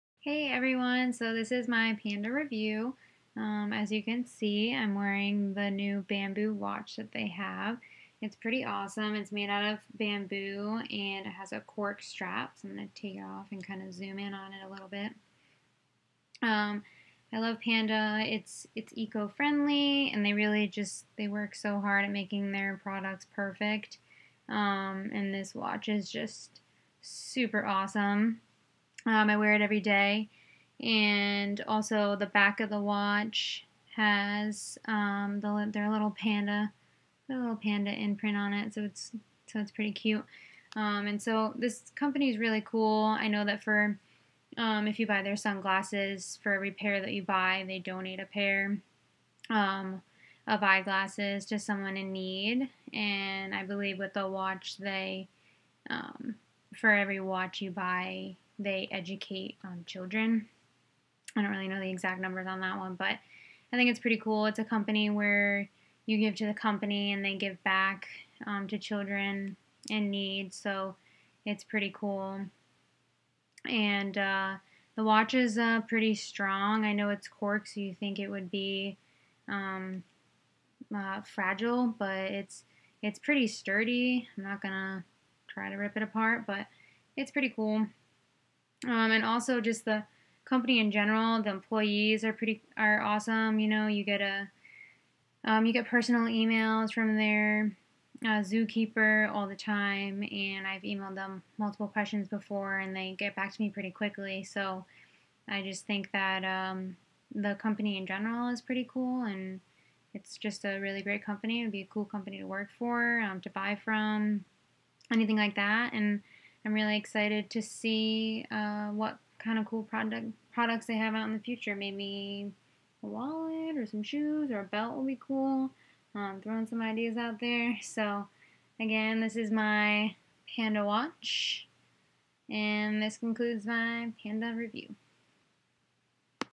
I love my Panda Bamboo watch! (wooden sunglasses company)
Panda is an awesome company that makes bamboo watches and sunglasses. They are an amazing company with quality product!! If you do not own this yet, go for their products and information!